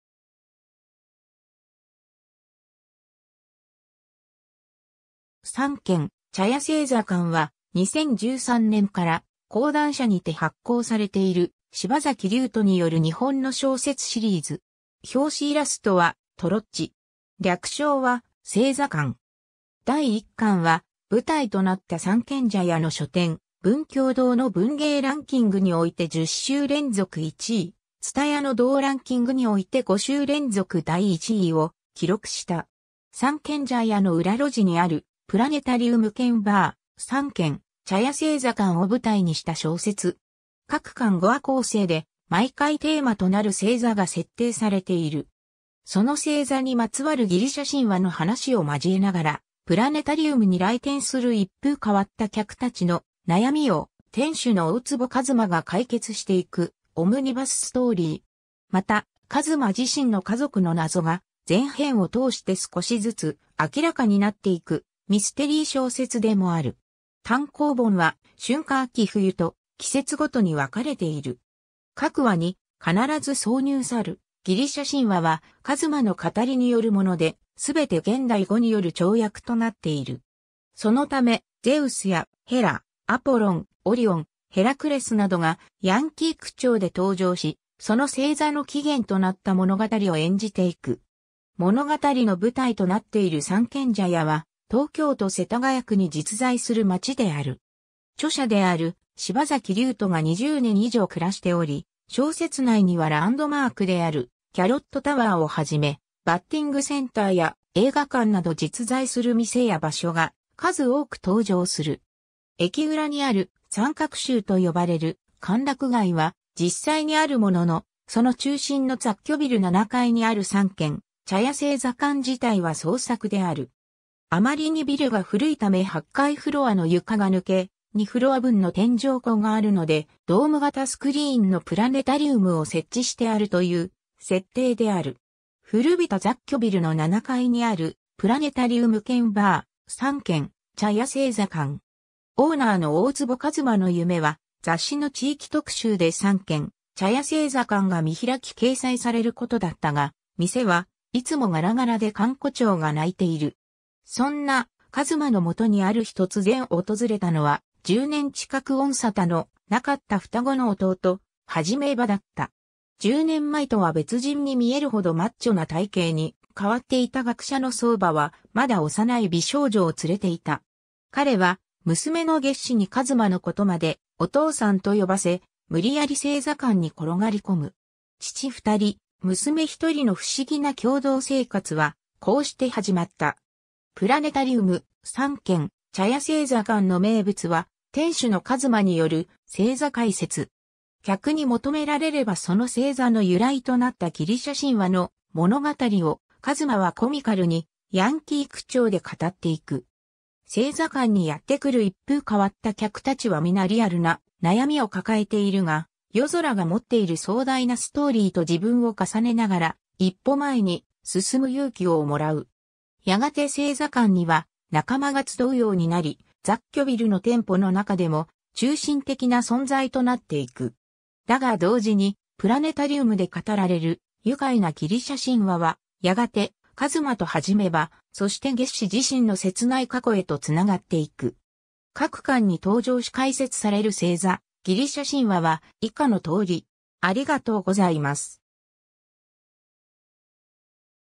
三軒茶屋星座館
『三軒茶屋星座館』（さんげんぢゃやせいざかん）は、2013年から講談社にて発行されている柴崎竜人による日本の小説シリーズ。
表紙イラストはとろっち。
略称は『星座館』。
 第1巻は舞台となった三軒茶屋の書店、文教堂の文芸ランキングにおいて10週連続1位、ツタヤの同ランキングにおいて5週連続第1位を記録した。
 三軒茶屋の裏路地にあるプラネタリウム兼バー「三軒茶屋星座館」を舞台にした小説。
各巻5話構成で、毎回テーマとなる星座が設定されている。
その星座にまつわるギリシャ神話の話を交えながら、プラネタリウムに来店する一風変わった客たちの悩みを店主の大坪和真が解決していくオムニバスストーリー。
また和真自身の家族の謎が全篇を通して少しずつ明らかになっていくミステリー小説でもある。
 単行本は春夏秋冬と季節ごとに別れている。
 各話に必ず挿入さるギリシャ神話は和真の語りによるもので、すべて現代語による超訳となっている。
そのためゼウスやヘラ、アポロン、オリオン、ヘラクレスなどがヤンキー口調で登場し、その星座の起源となった物語を演じていく。
 物語の舞台となっている三軒茶屋は、東京都世田谷区に実在する街である。
著者である柴崎竜人が20年以上暮らしており、小説内にはランドマークであるキャロットタワーをはじめ、バッティングセンターや映画館など実在する店や場所が数多く登場する。
 駅裏にある三角州と呼ばれる歓楽街は実際にあるものの、その中心の雑居ビル7階にある三軒茶屋星座館自体は創作である。
あまりにビルが古いため8階フロアの床が抜け、2フロア分の天井高があるのでドーム型スクリーンのプラネタリウムを設置してあるという設定である。
 古びた雑居ビルの7階にあるプラネタリウム兼バー「三軒茶屋星座館」。
オーナーの大坪和真の夢は、雑誌の地域特集で三軒茶屋星座館が見開き掲載されることだったが、店はいつもガラガラで閑古鳥が鳴いている。
 そんな和真のもとにある日とつぜん訪れたのは、10年近く音沙汰のなかった双子の弟・創馬だった。
10年前とは別人に見えるほどマッチョな体型に変わっていた学者の創馬は、まだ幼い美少女を連れていた。
彼は娘の月子に和真のことまで「お父さん」と呼ばせ、むりやり星座館に転がり込む。
父2人、娘ひとりの不思議な共同生活はこうしてはじまった。
 プラネタリウム・三軒茶屋星座館の名物は店主の和真による星座解説。
客に求められればその星座の由来となったギリシャ神話の物語を、和真はコミカルにヤンキー口調で語っていく。
星座館にやってくる一風変わった客たちはみなリアルな悩みを抱えているが、夜空が持っている壮大なストーリーと自分を重ねながら、一歩前に進む勇気をもらう。
やがて星座館には仲間が集うようになり、雑居ビルの店舗のなかでも中心的な存在となっていく。
 だが同時に、プラネタリウムで語られる愉快なギリシャ神話は、やがて和真と創馬、そして月子自身の切ない過去へと繋がっていく……。
 各巻に登場し解説される星座 / ギリシャ神話は以下の通り。